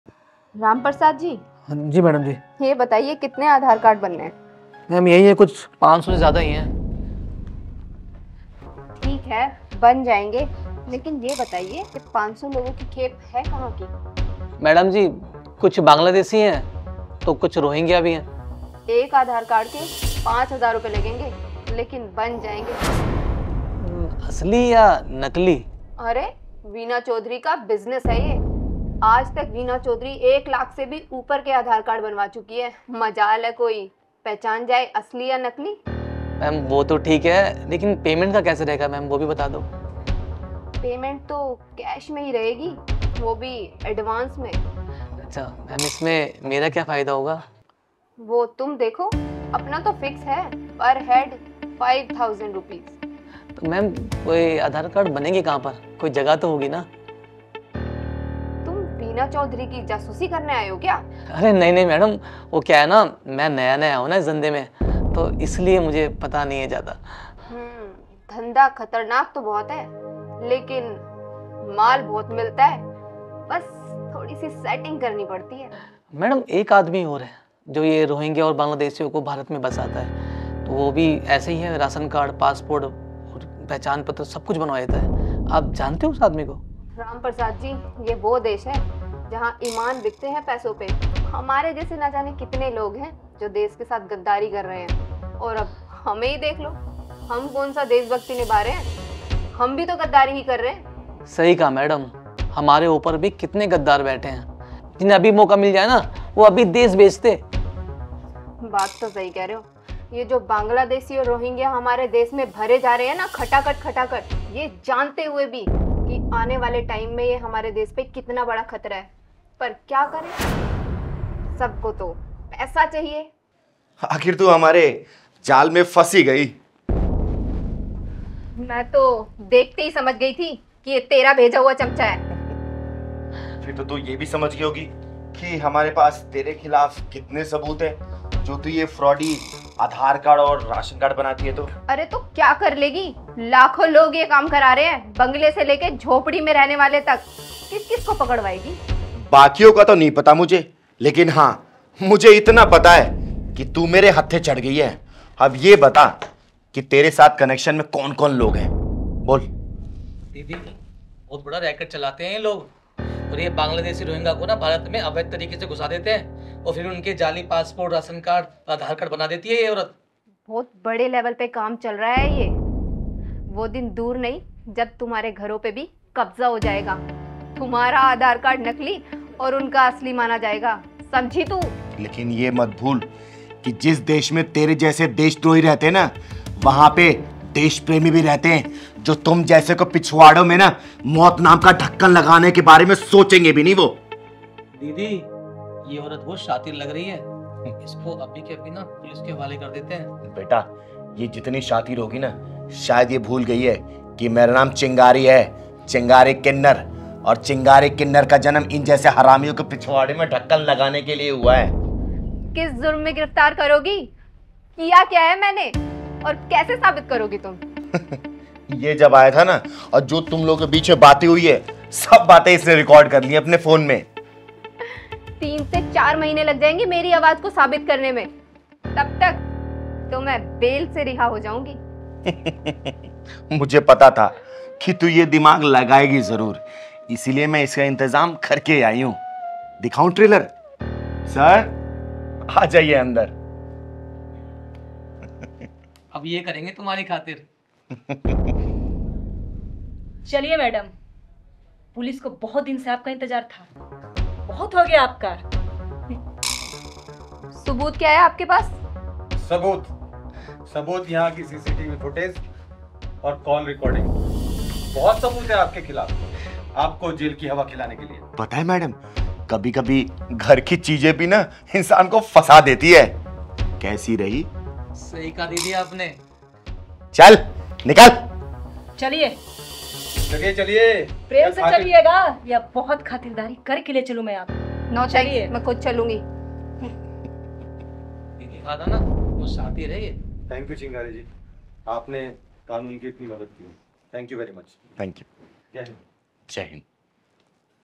चिंगारी किन्नर ने पकड़ा नकली आधार कार्ड वाले ग्रुप को | Ujjval Dehati | Chingari Kinner New Video
Story & Screenplay :- Sukdev Nishad
Director :- Pawan Pathak
Cast :- Aditya Giri
Camera Man :- Sonu Paji, Rajesh Mahto
Makeup :- Gunjan Panchal
Editor :- Sadik, Kailash, Kuldeep
Label :- Ujjval Dehati